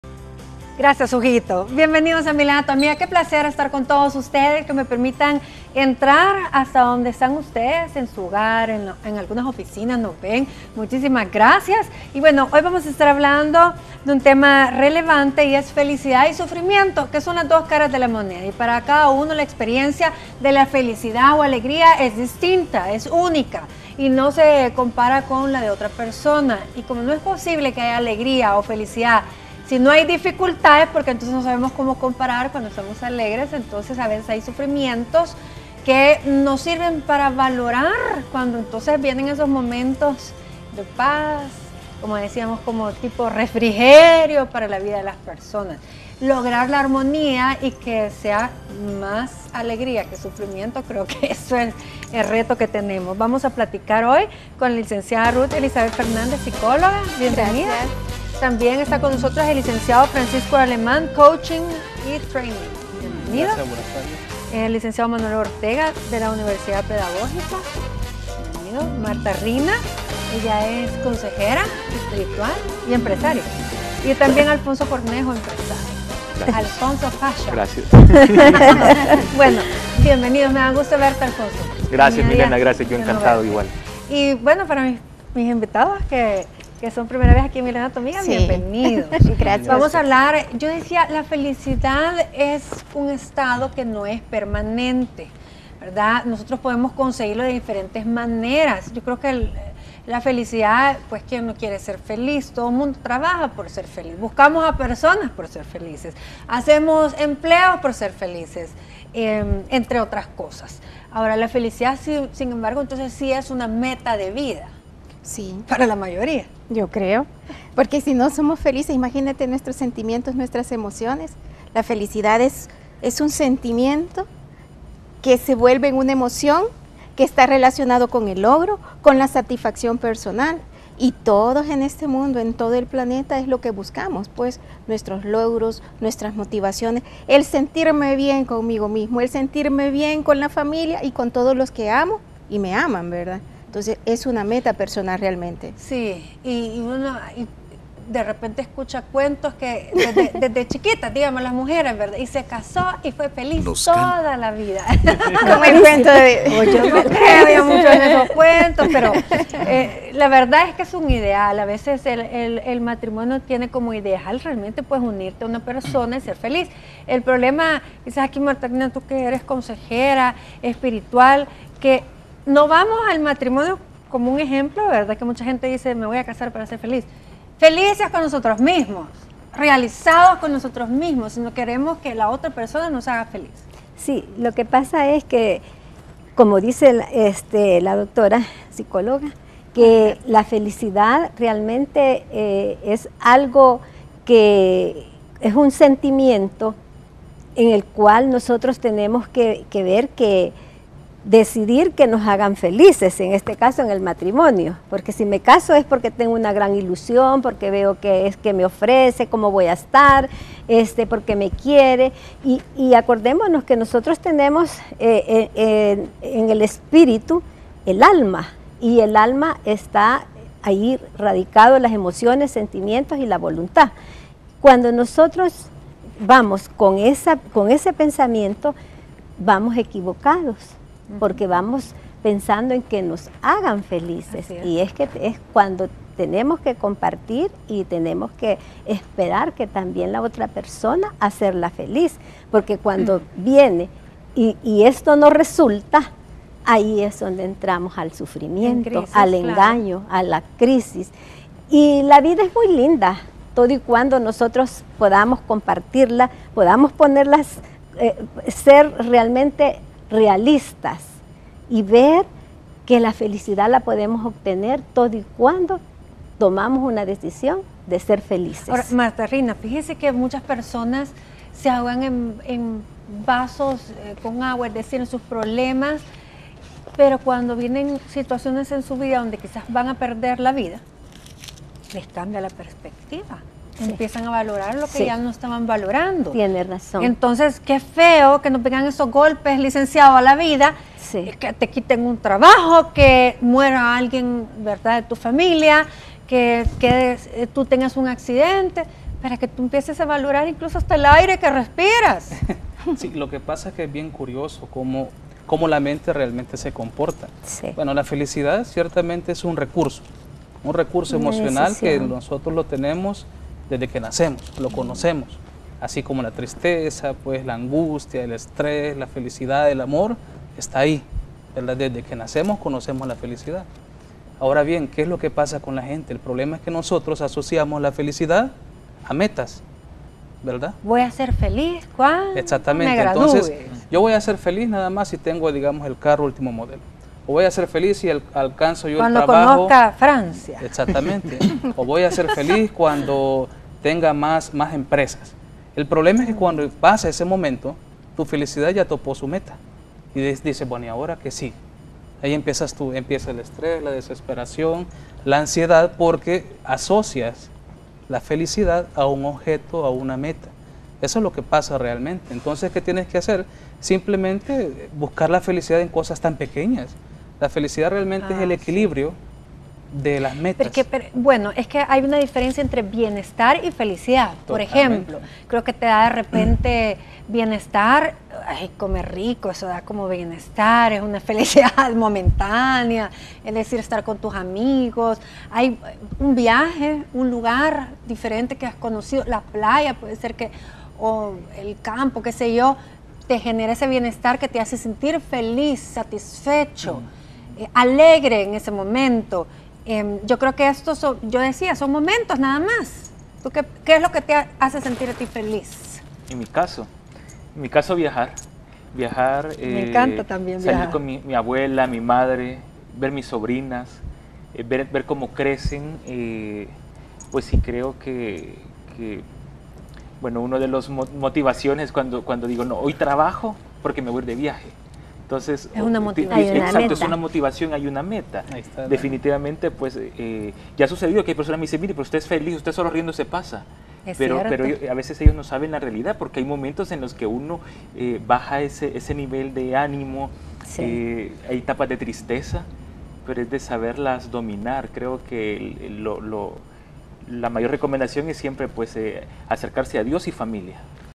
Las dos caras de la moneda: Felicidad y Sufrimiento
La felicidad es una emoción que se produce en la persona cuando cree haber alcanzado una meta deseada. Está aparejada a una condición interna o subjetiva de satisfacción y alegría. En cambio, el sufrimiento es la sensación motivada por cualquier condición que someta a un sistema nervioso al desgaste.